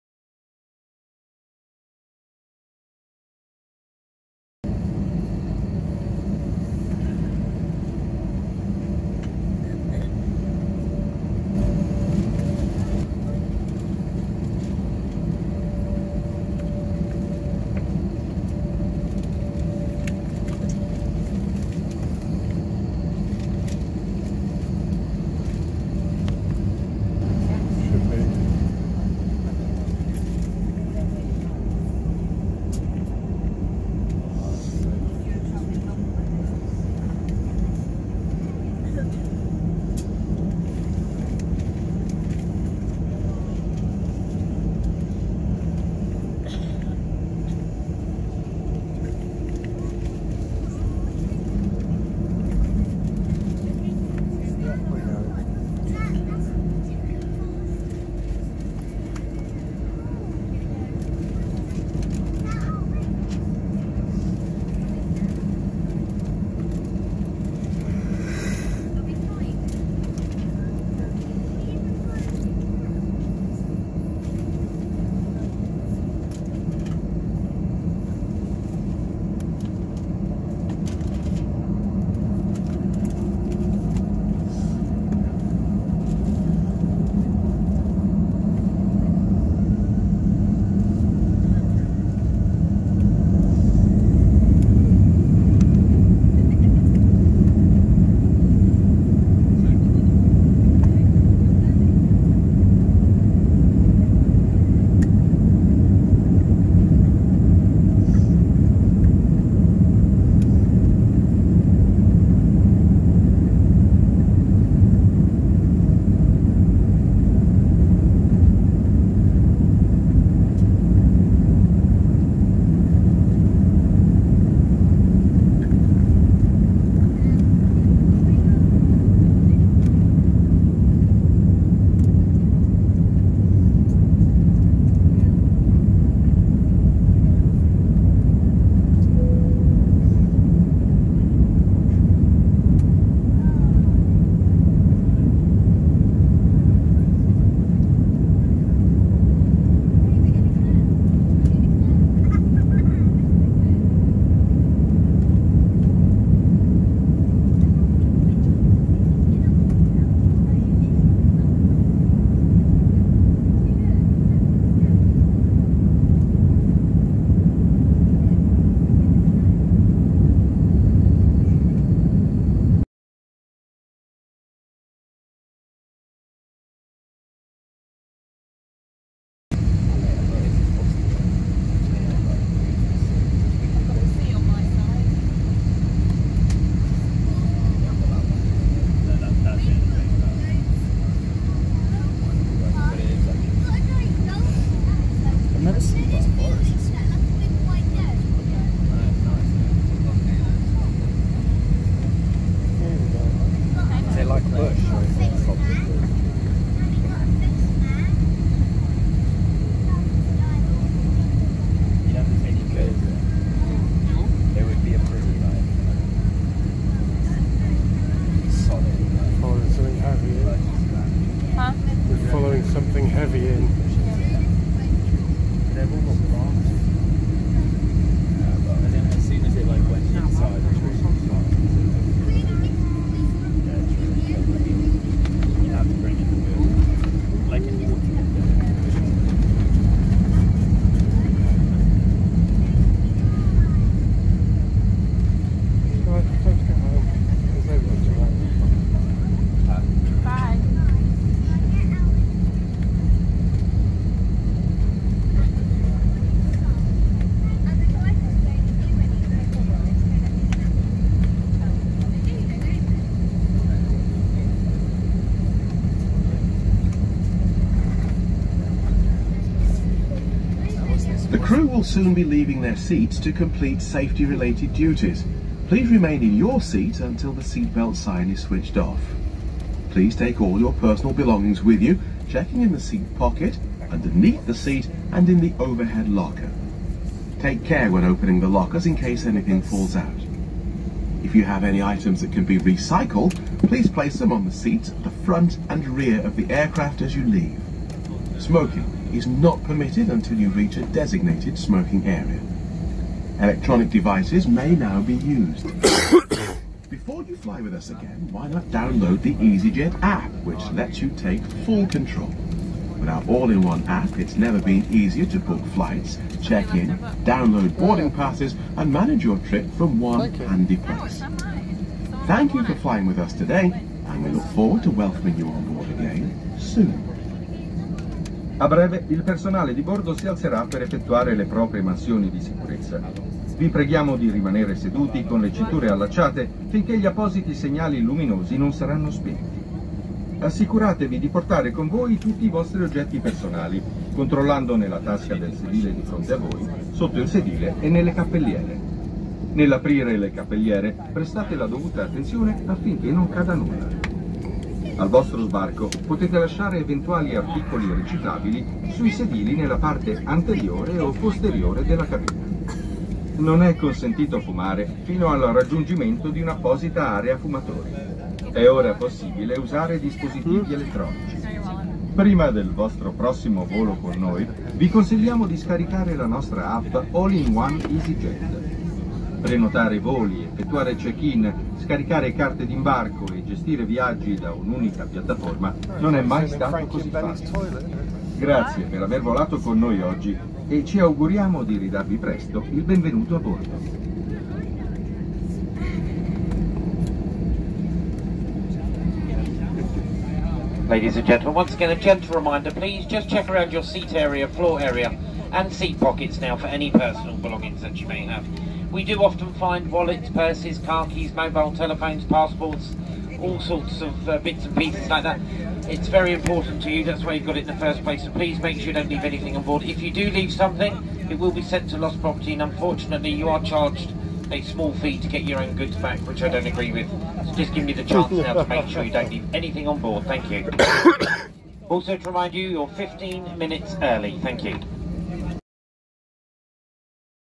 Gatwick to Rome Easy Jet A319 G-EZGF
A quick trip to Rome resulted in my first Easy Jet flight - we were on an A319 from LGW / EGKK to FCO/LIRF (G-EZGF).
I made a rookie mistake of not checking the seats online first and went for the very back row window seat - without a window! It made it feel really claustrophobic and you are relying on the person in front to keep the blind open - which they didnt!
the flight was pretty uneventful - but the city of Rome is WELL worth a visit! amazing place!